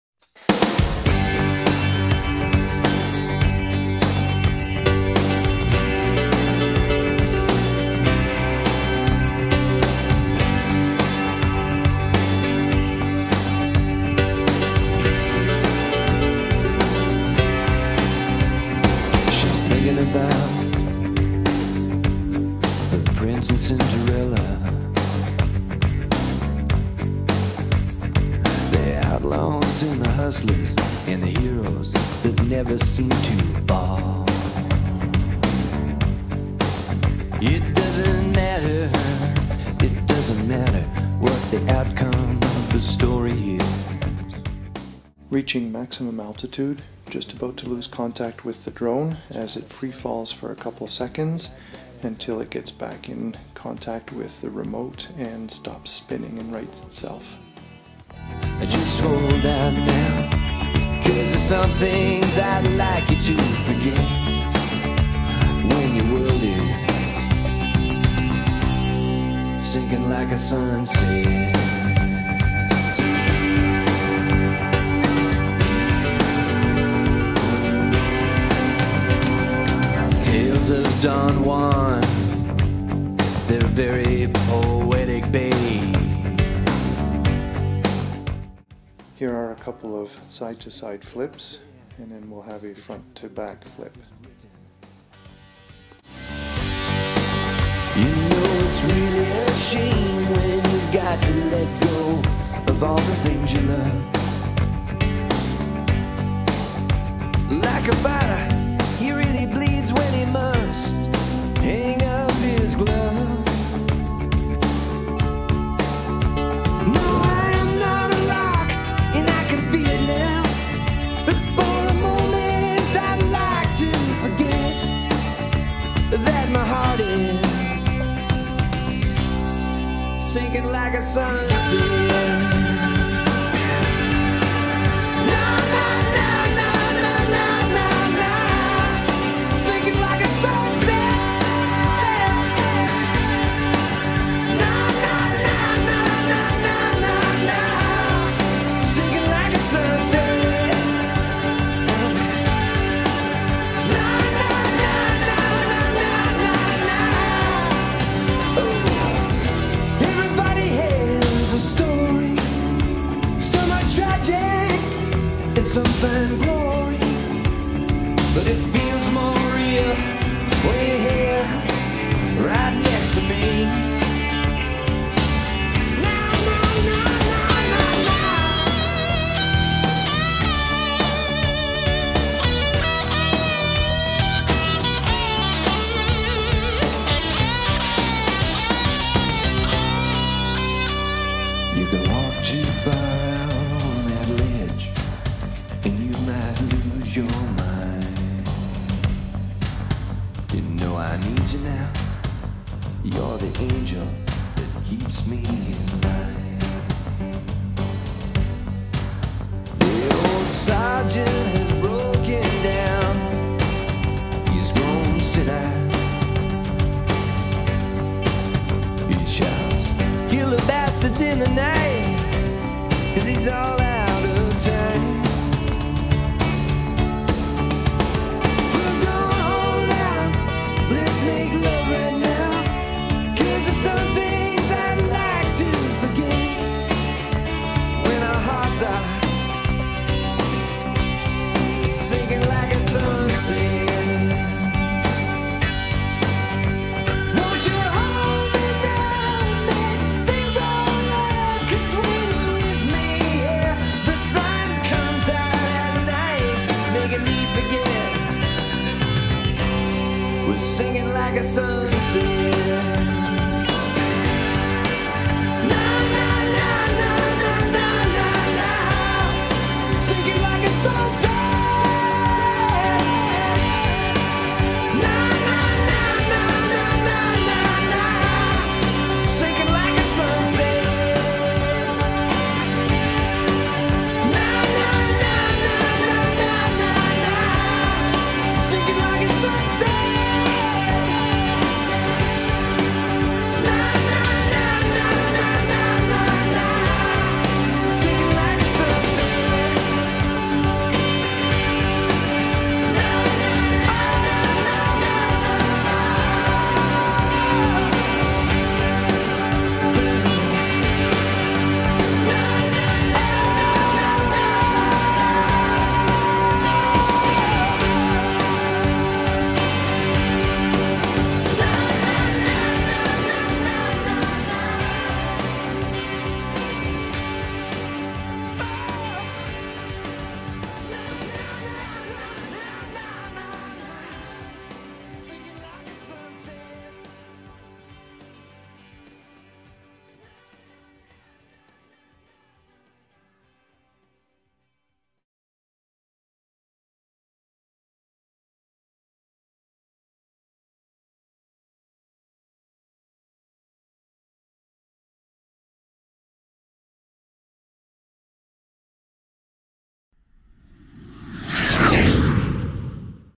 X6SW Max Altitude-loss of comms-free fall save
Going to max altitude, lost comms with the drone, if free fell for a couple seconds and reconnected